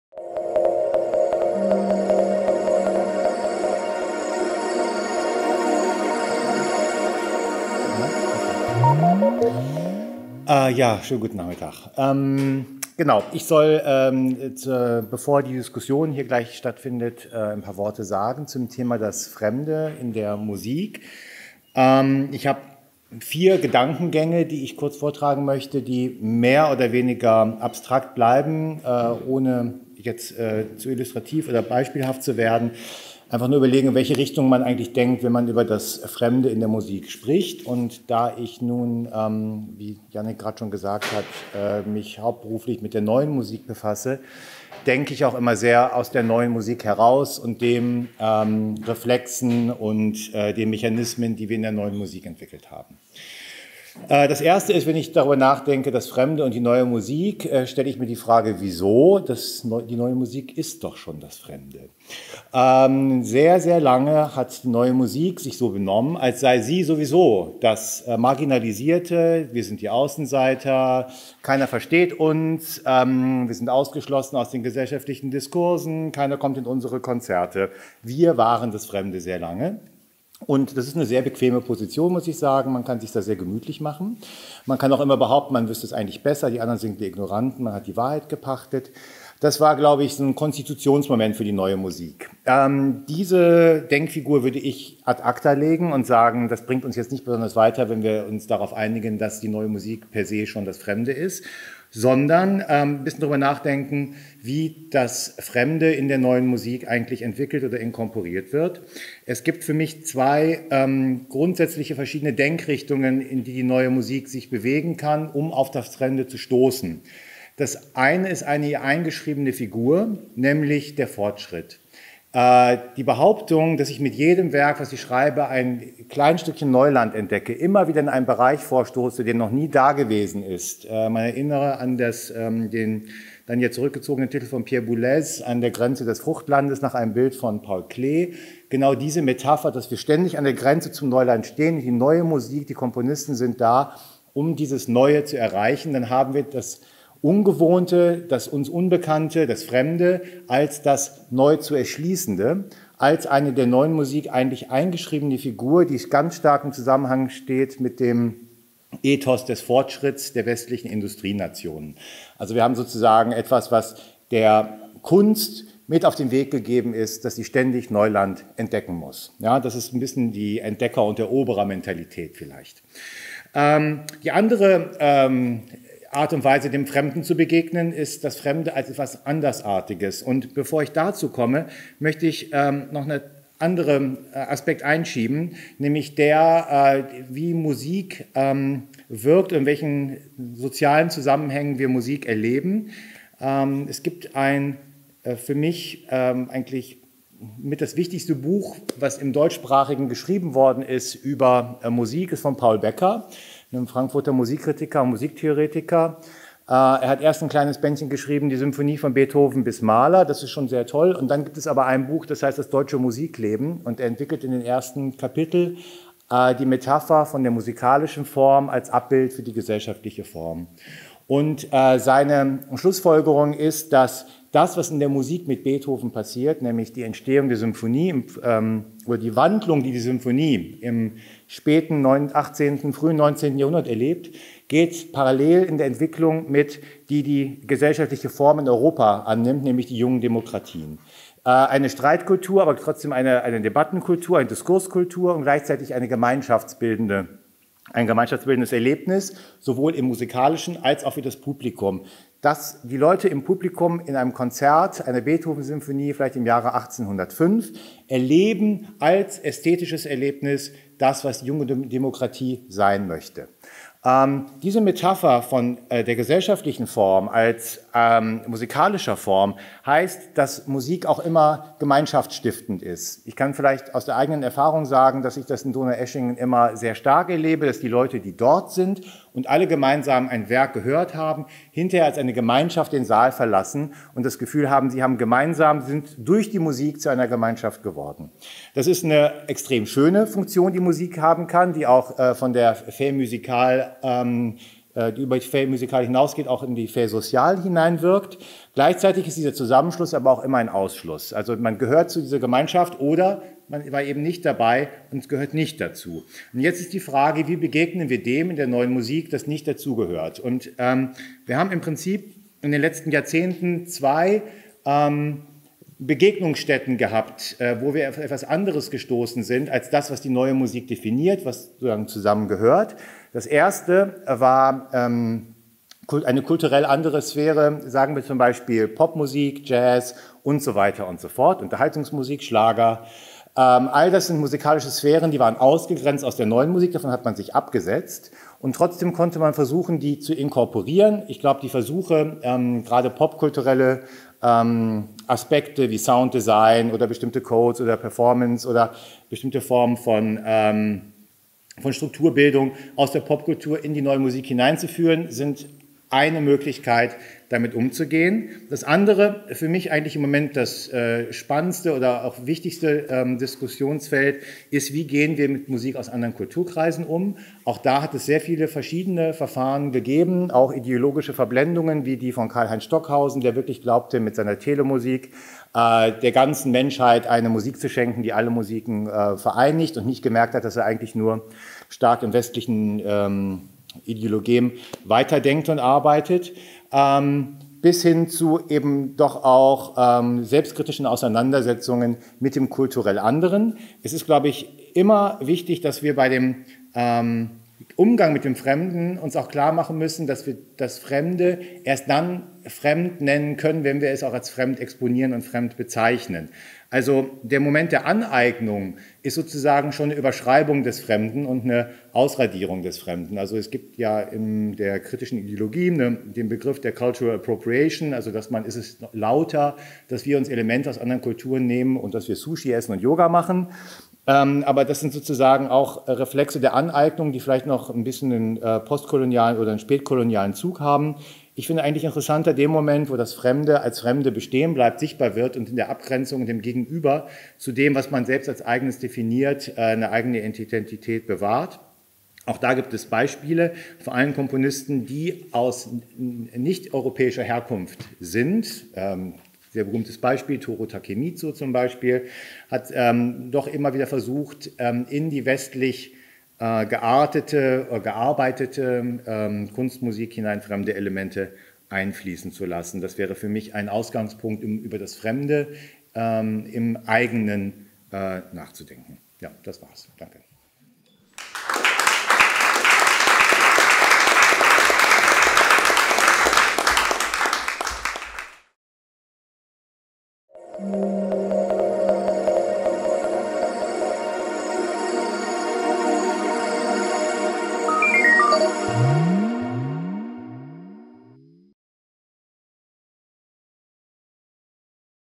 Björn Gottstein | Impuls: Das Fremde in der Musik
Mit Diskussionen, Performances und Konzertbeiträgen gibt der bebeethoven-Showcase einen Einblick in das Fellowship-Programm bebeethoven, das anlässlich des Beethoven-Jubiläums im Jahr 2020 initiiert wurde. Wie kommt die Kunst in die Kultur? Wie kommt das Fremde in die Musik? Wie gehen wir künstlerisch mit neuen Dimensionen wie Künstlicher Intelligenz um? Im Rahmen des Showcases diskutieren die KünstlerInnen des Programms mit VertreterInnen der Partnerinstitutionen und des ZKM, dem Kurator von bebeethoven Steven Walter sowie dem Publikum über diese zentralen Fragen des künstlerischen Musikschaffens.

Videodokumentation:
ZKM | Videostudio

Kamera: Christina Zartmann, Lisa Baumgärtner
Schnitt: Tim Wenz